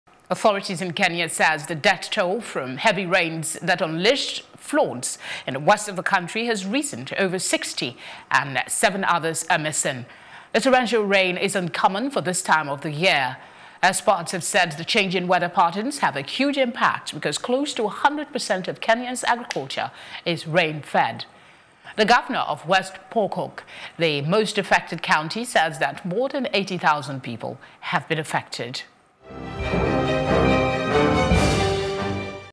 Kenya Flooding: Death Toll Increases To 65
Authorities in Kenya says the death toll from heavy rains that unleashed floods in the west of the country has risen to over 60 and seven others are missing.

The torrential rain is uncommon for this time of year. Experts have said the changing weather patterns have a huge impact because close to 100% of Kenya’s agriculture is rain-fed.

The governor of West Pokot, the most affected county, says that more than 80,000 people have been affected.